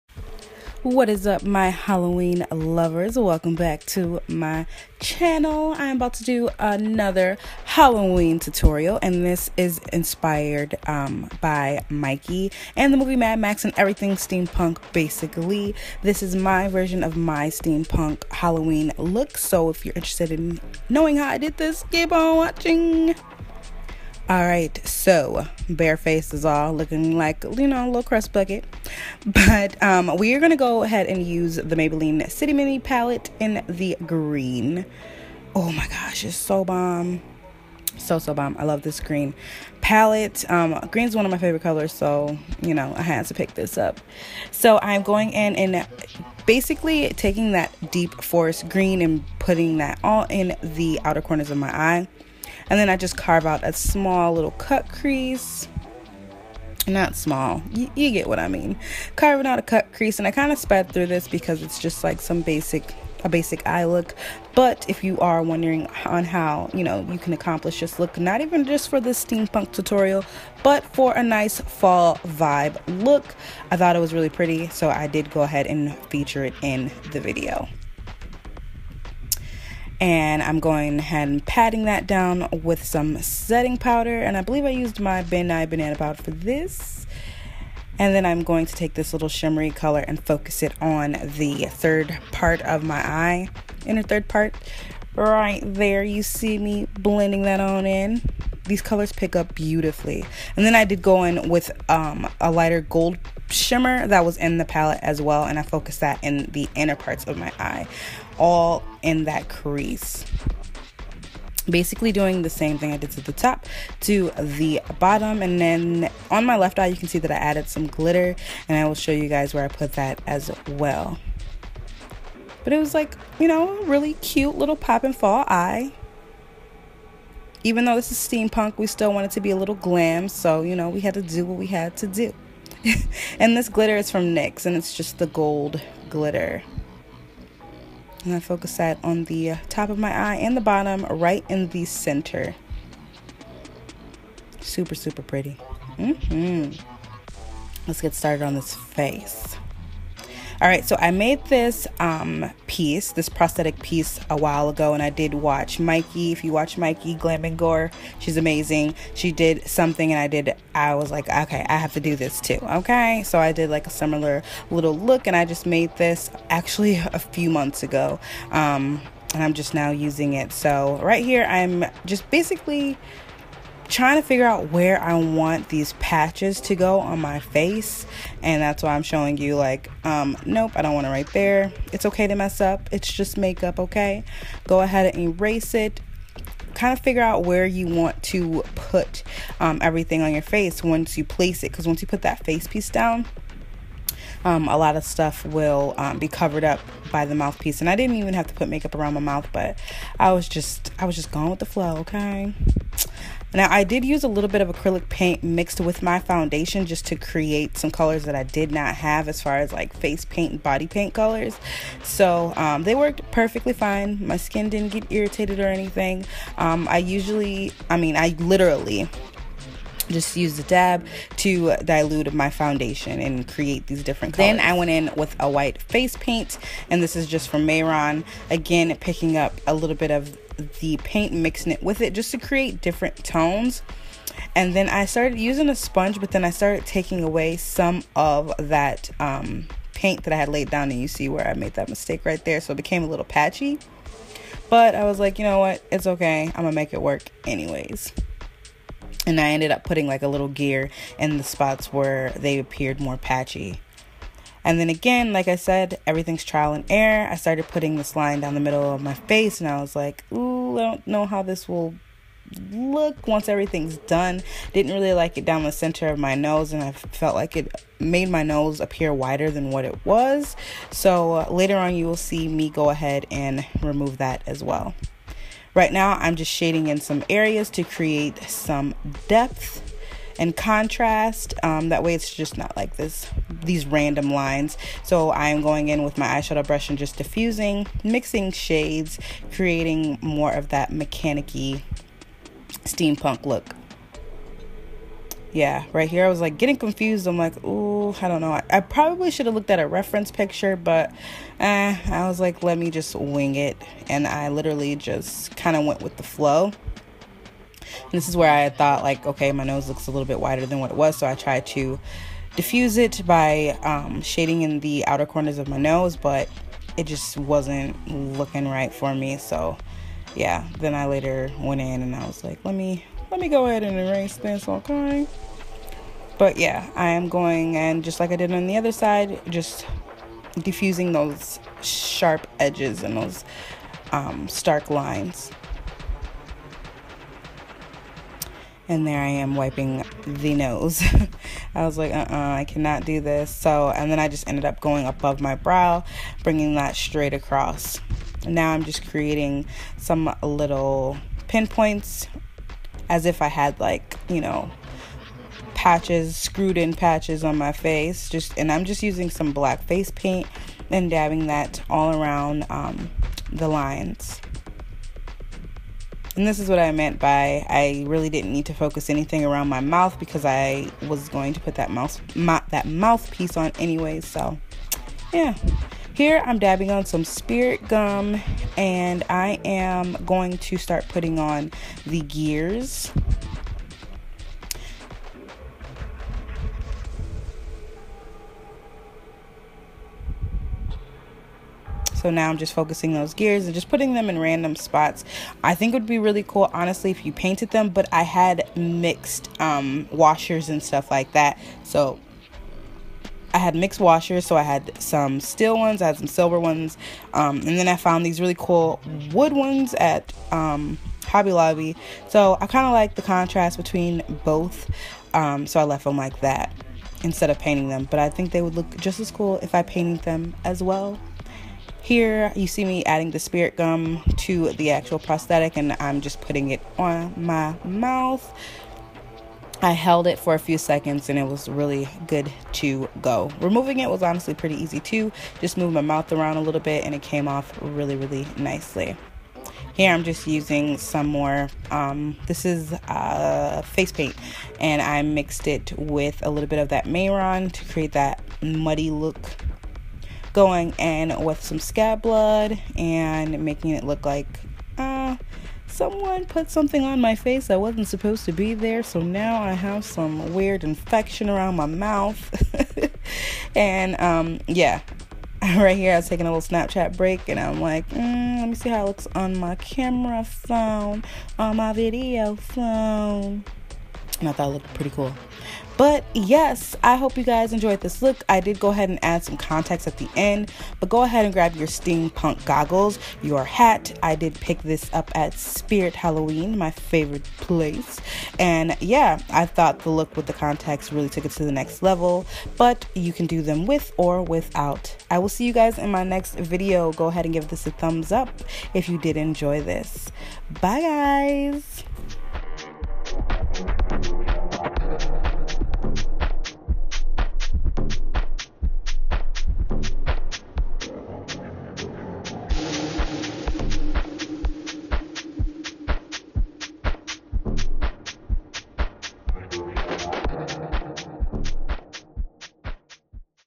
STEAMPUNK MAKEUP TUTORIAL 2017 HALLOWEEN
HALLOWEEN IS MY FAVORITE HOLIDAY ! I HAVE BEEN WAITING SOOOOO LONG TO DO THIS STEAMPUNK MAKEUP LOOK! I GOT SOME INSPO FOR THIS DOPE MOUTH PIECE FROM MYKIE AKA GLAM N GORE!!! THUMBS UP IF YOU ENJOYED THIS VIDEO!!